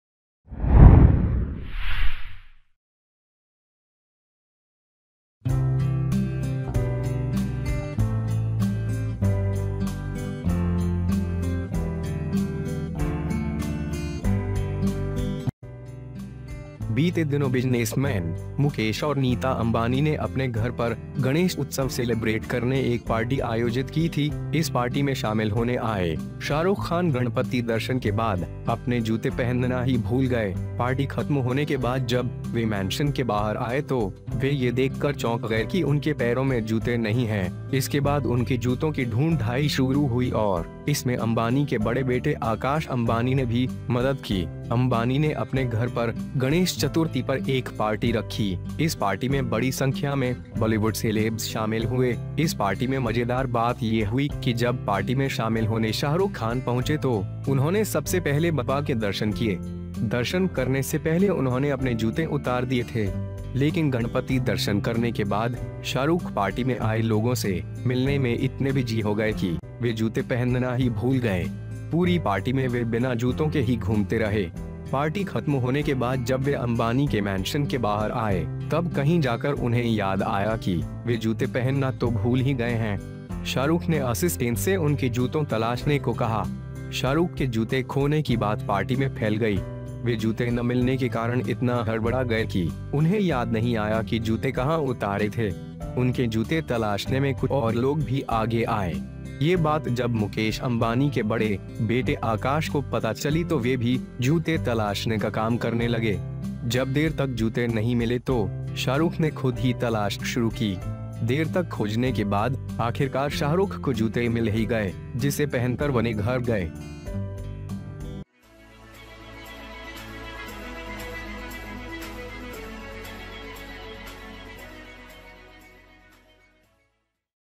गणेश दर्शन के बाद जूते पहनना भूले शाहरुख, गुम हुए तो अंबानी ने ढूंढे और..
In the past, businessmen Mukesh and Nita Ambani organized a party to celebrate Ganesh festival at their home. Shah Rukh Khan, who came to join this party, forgot to wear his shoes after Ganapati philosophy. After the party ended, when they came out of the mansion, they were shocked to see that there was no shoes in their feet. After this, their shoes began to be found and Ambani's eldest son, Akash Ambani, also helped. Many Selebes were in the party ...

Ambani placed a party on Ganesh Chaturthi at his house. A large number of Bollywood celebs were involved in this party. The funny thing about this party was that when Shahrukh Khan joined party, he first appeared in Bappa's eyes. He had dropped his shoes before appearing. But after visiting Ganpati, Shahrukh became so busy in meeting the people who came in the party that they forgot to wear shoes. In the whole party, they roamed without shoes.

 After the party's end, when he came out of Ambani's mantra, he went somewhere and remembered that he had forgotten to wear shoes. Shahrukh asked the assistant to find his shoes. The talk of losing Shah Rukh's shoes was spread in the party. They were so stunned because they did not get the shoes that they did not remember where the shoes were rolled. Some people came forward to explore their shoes too. When this thing came to know that Mukesh Ambani's eldest son, Akash, he also started working to find shoes. When the shoes did not get long, Shahrukh started looking for himself. After searching for a long time, the shoe finally got to Shah Rukh, who wore to go home.

HJ NEWS !!